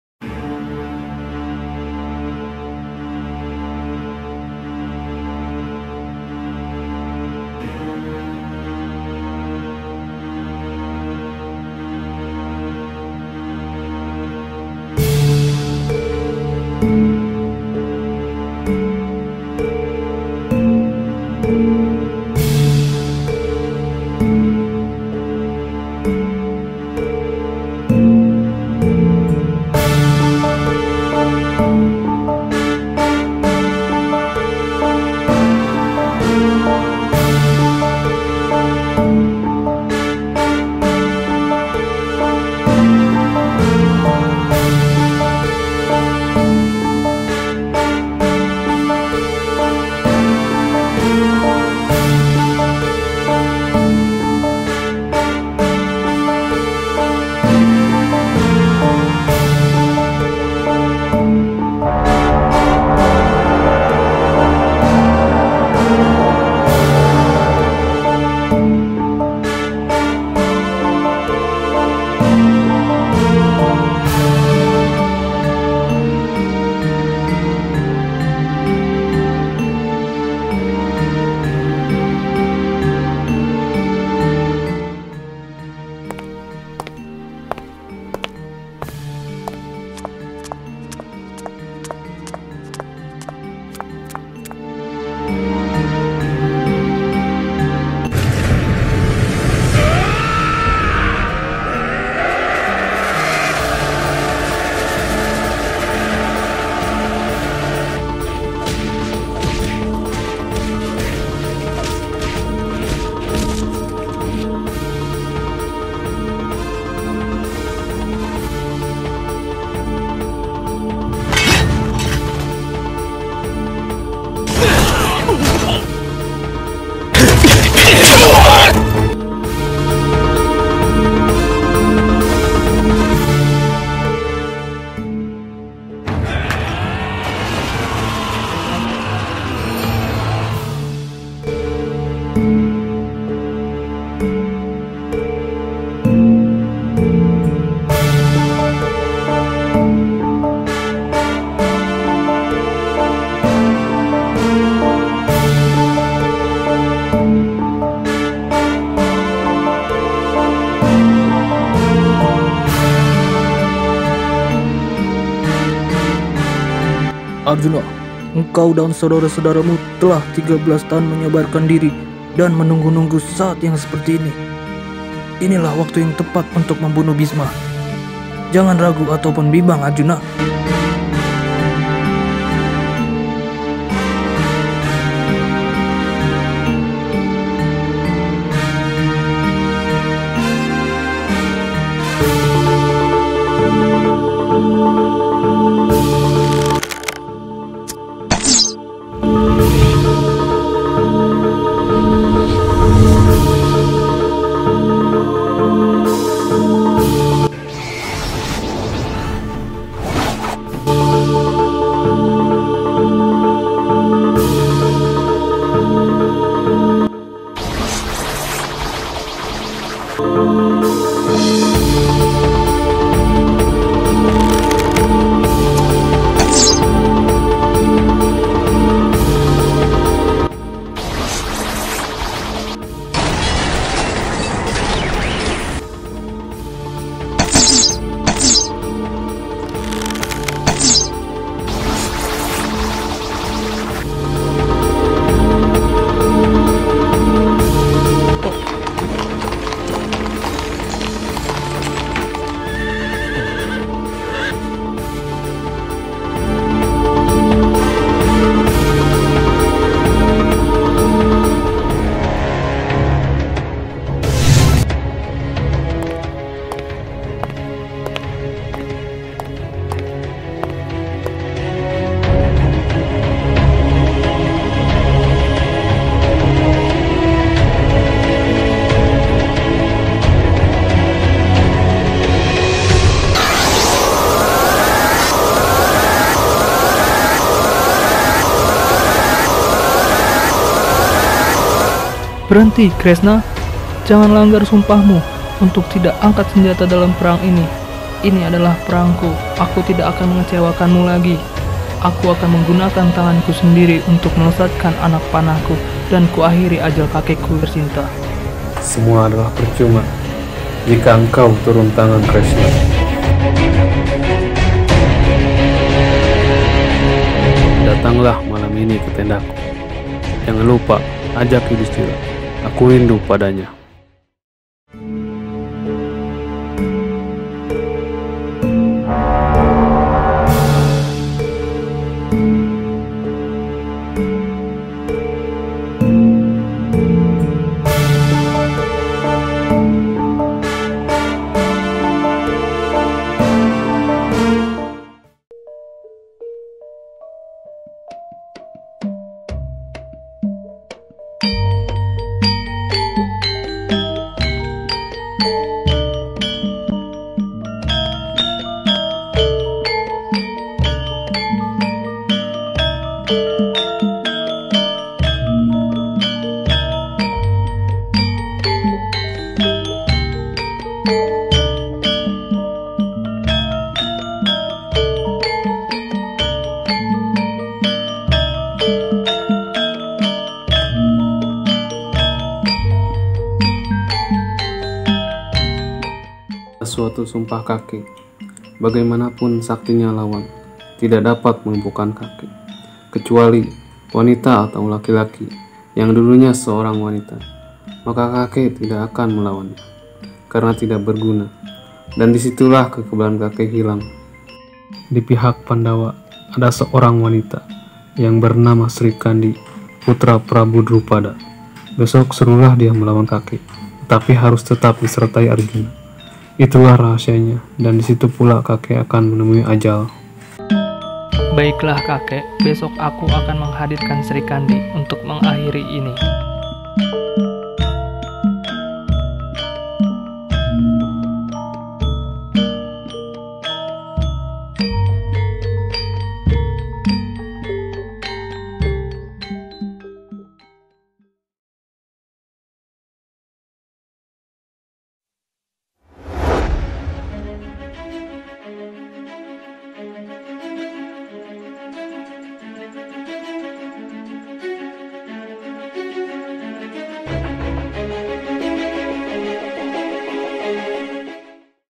Film Animasi Perang Baratayudha Hari Ke-9 Sebagai Sikap Keteguhan Hati Dalam Berperang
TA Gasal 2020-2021
Nama : Rizki Dwi Siswanto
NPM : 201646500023
Judul : Film Animasi Perang Baratayudha Hari Ke-9 Sebagai Sikap Keteguhan Hati Dalam Berperang